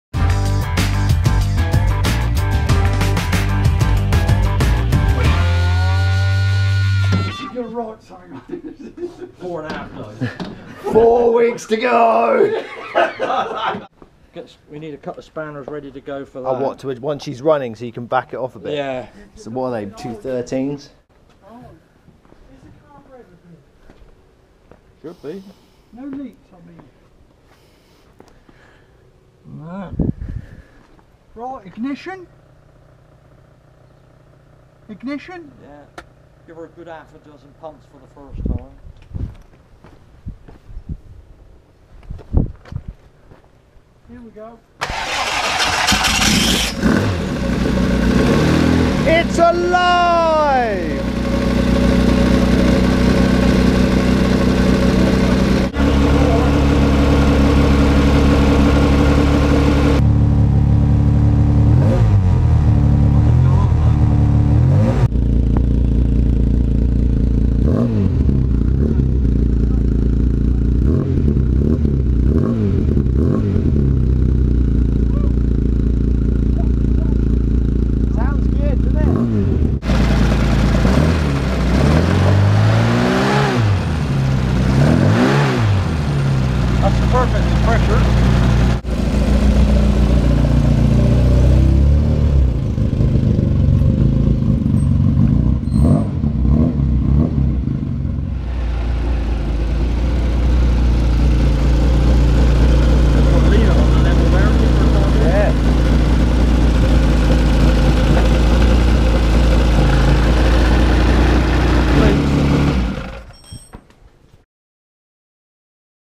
BST 4 weeks to go
The boys from Dutchspeed fire up the new race engine